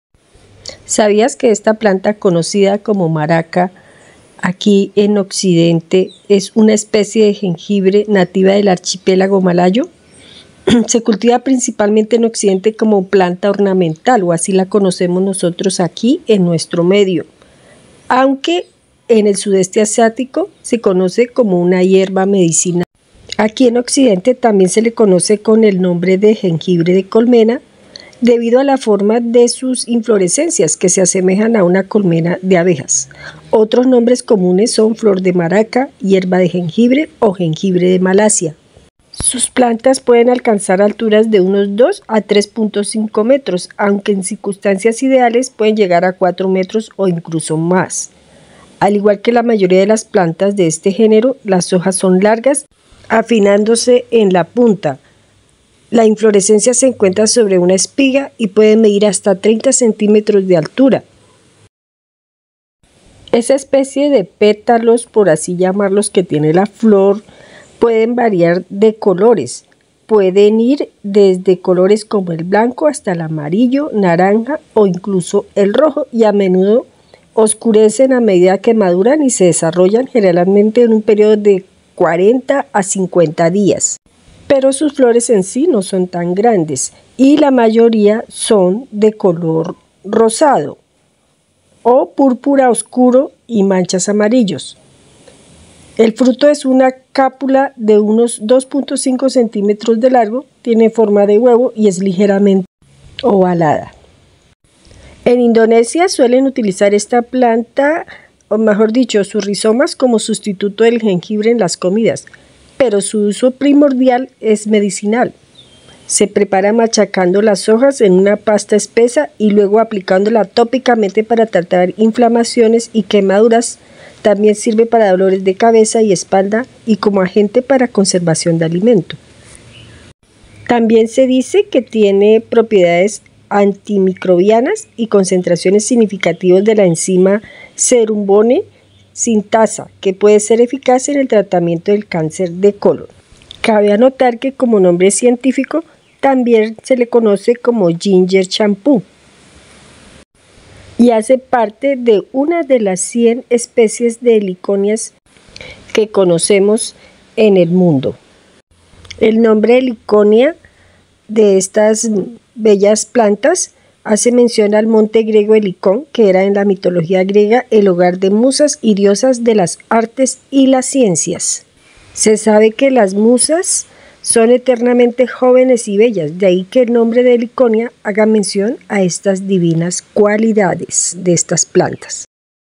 Heliconeas: curiosidades de esta variedad
Enterate de las características de estas plantas sus beneficios en la salud la gastronomía y como decoración así como de sus nombres y colores entre otras